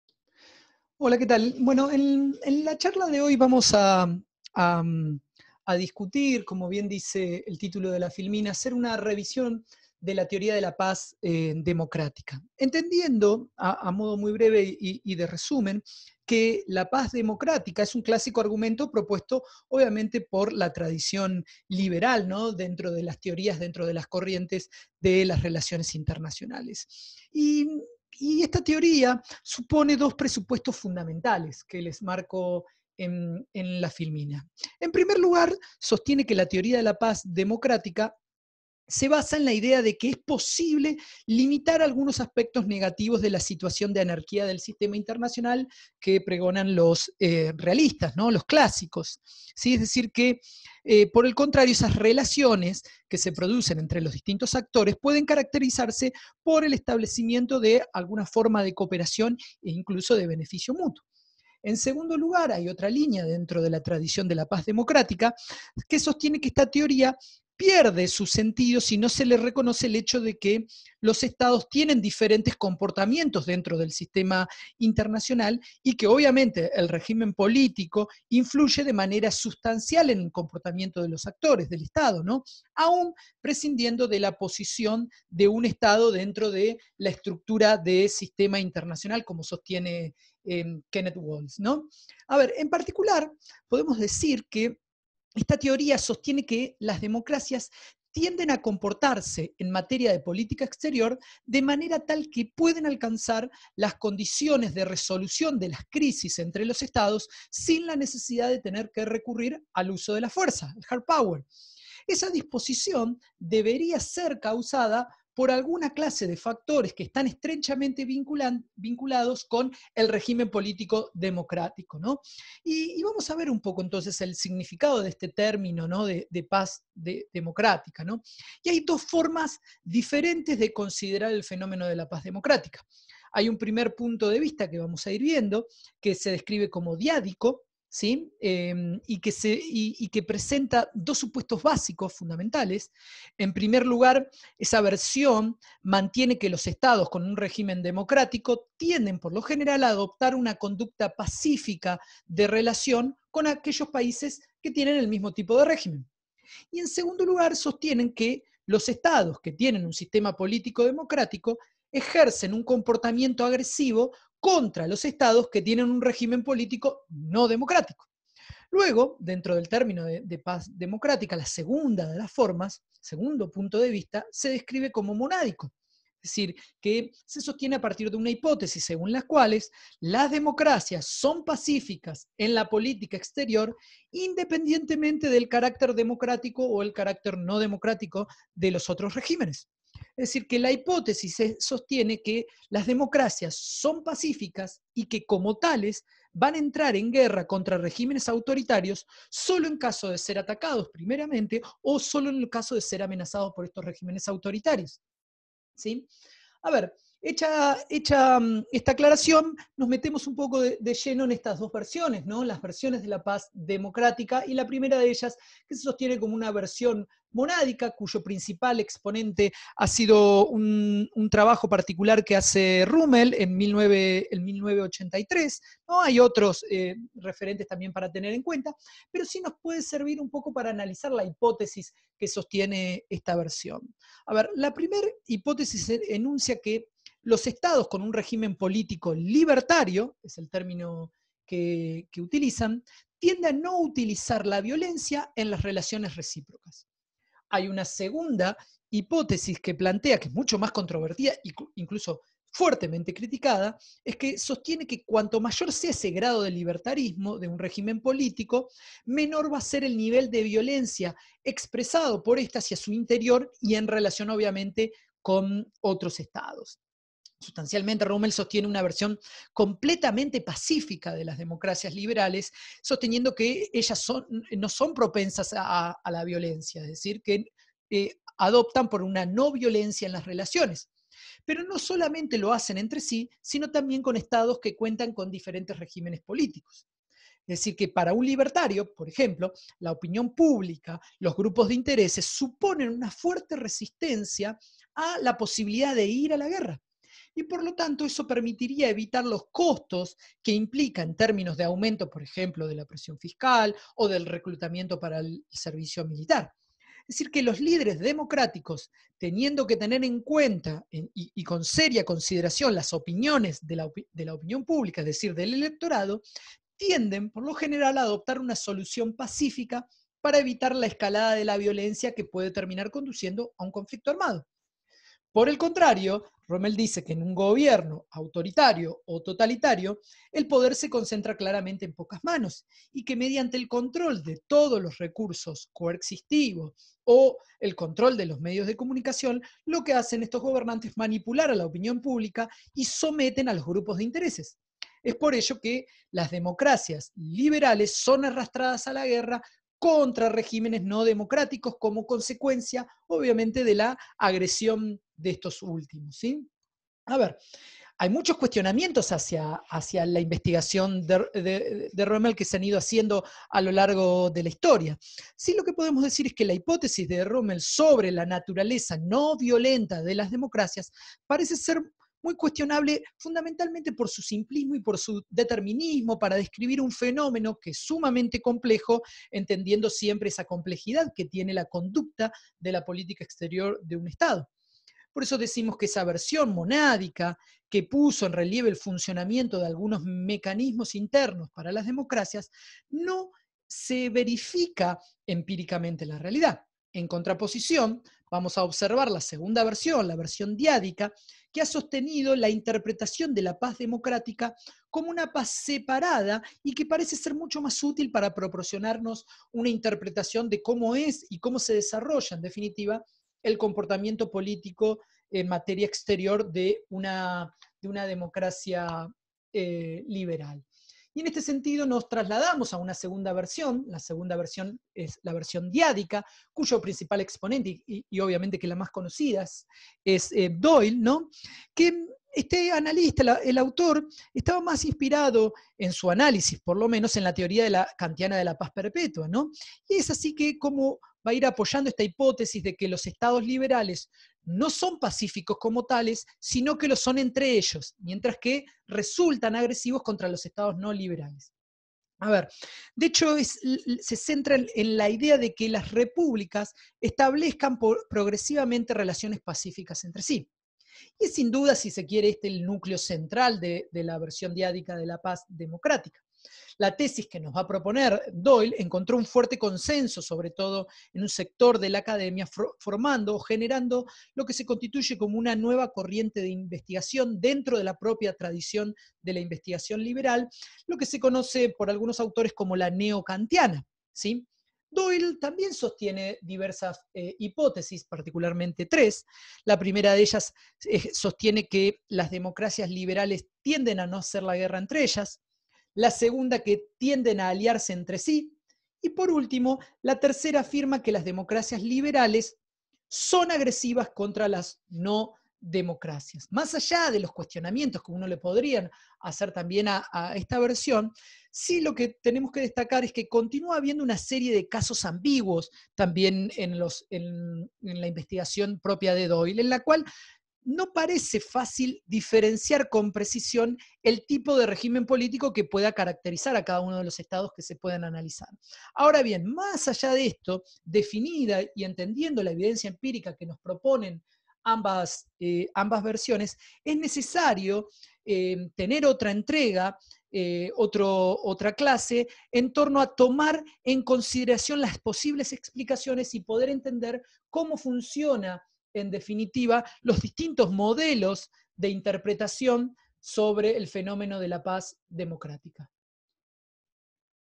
Una revisión de la teoría de la Paz Democrática.
El debate sobre la «tesis de la paz democrática» ha hecho correr ríos de tinta en los últimos años, sobre todo, aunque no exclusivamente, en el ámbito académico estadounidense de las Relaciones Internacionales. Tradicionalmente, los autores liberales han señalado la existencia de un vínculo entre democracia y paz. Desde hace unos quince años, un grupo bastante numeroso de académicos ha pasado a sostener que ese vínculo se ha podido demostrar empíricamente.